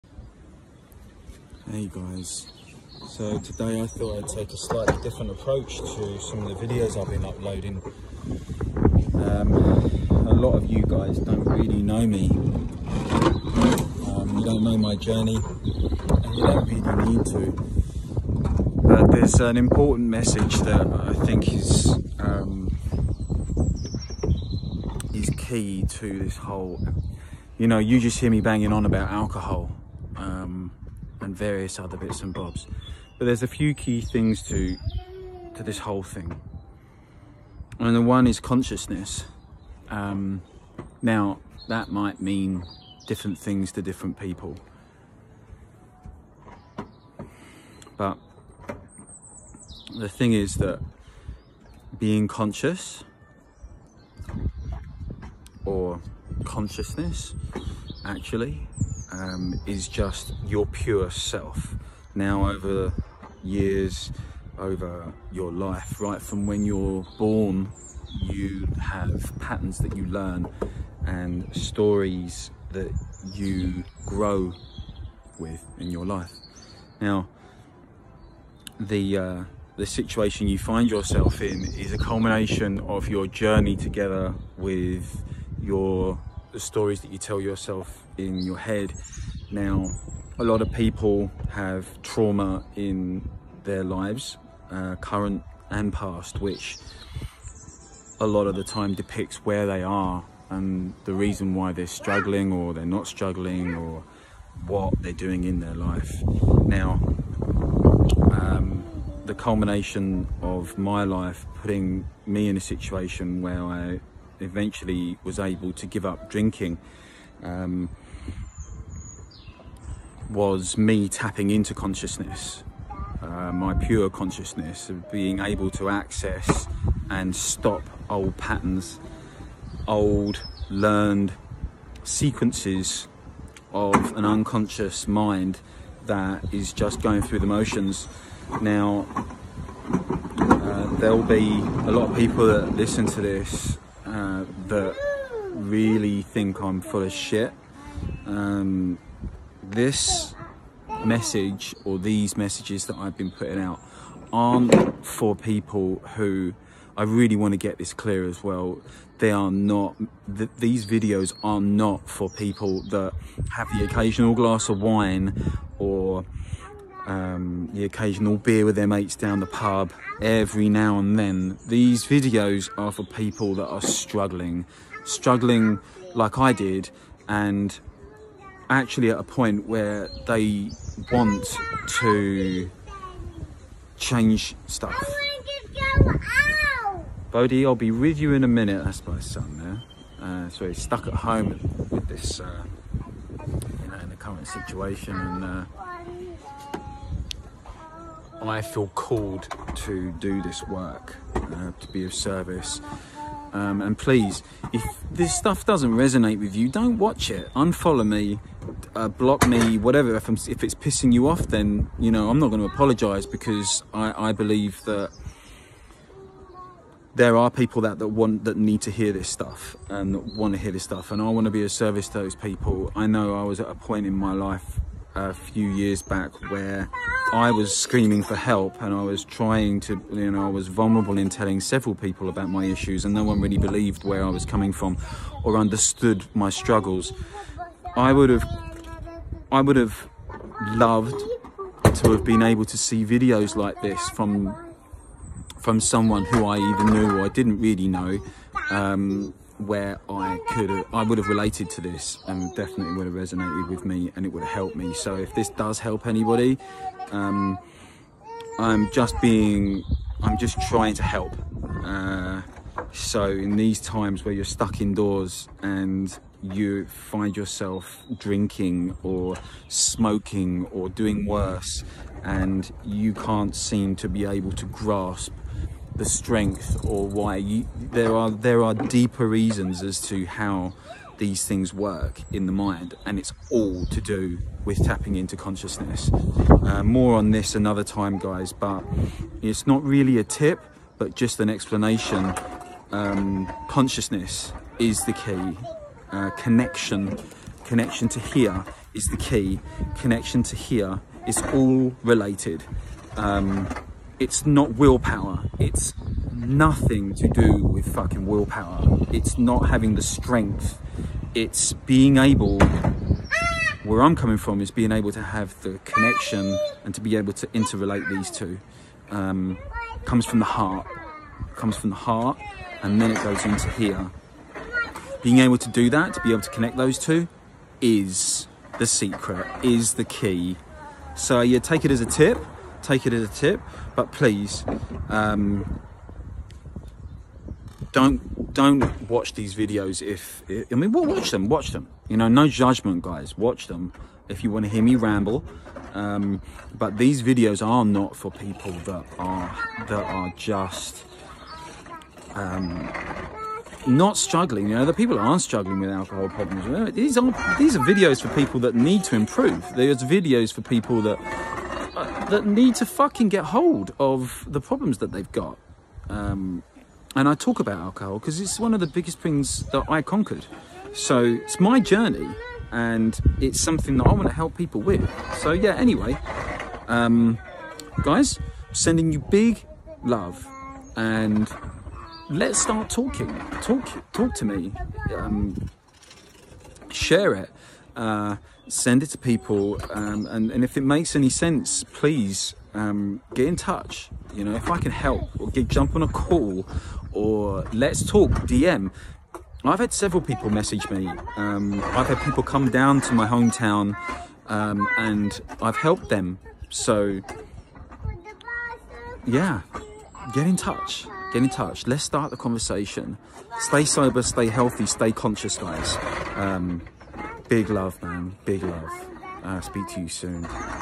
Consciousness vs willpower
It’s not will power that gets us through to the other side, it’s consciousness.
My current rambling daily  vlog.
Today I wanted to talk about who I am trying to reach and maybe let you see that it’s not just about Giving up alcohol’ it’s also a lot more.
I got you if you need to talk
Big love